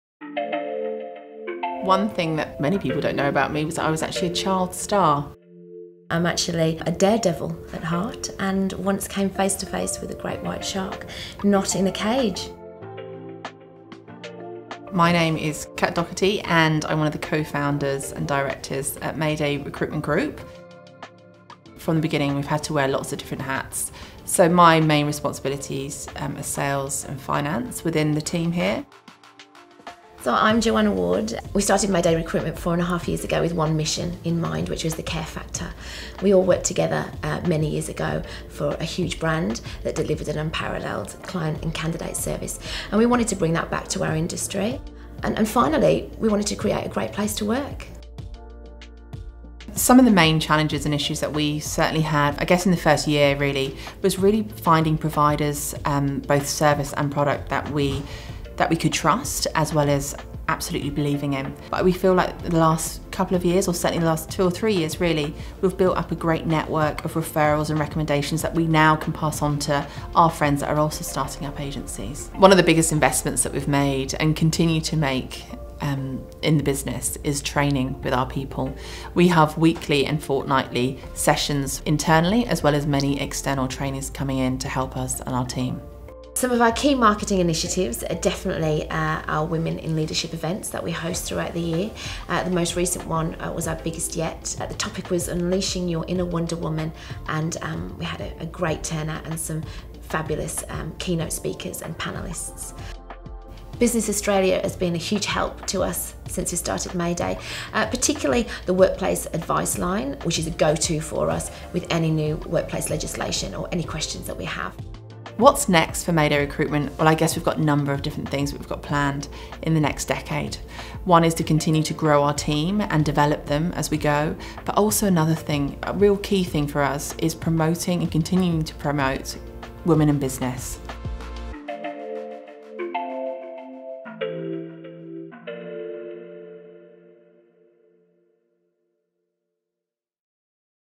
Opening new doors with MAYDAY Recruitment
When Co-Founders Kat Docherty and Joanna Ward started MAYDAY Recruitment with their friend Imogen Storie, they had one mission in mind – the care factor. Kat, Joanna and Imogen wanted to create a great place to work that delivered an unparalleled client and candidate experience.

MAYDAY Recruitment faced a number of issues when they first started out. One of the biggest was finding service and product providers they could trust, and absolutely believe in.

Over the last few years, the team have built up a great network of referrals and recommendations that they can now can pass on to their friends who are also starting up recruitment agencies.

One of the biggest investments Kat and Joanna have made, and continue to make, in their business is training. MAYDAY Recruitment run weekly internal training sessions with their employees, as well as bringing in external trainers when needed.

The team at MAYDAY Recruitment credit My Business as being huge help to the company since they started. They reply on our Workplace Advice Line as the go-to for any new workplace legislation updates.

MAYDAY Recruitment has a lot planned for the future. Kat and Joanna will continue to grow and develop their team and continuing to promote women in business.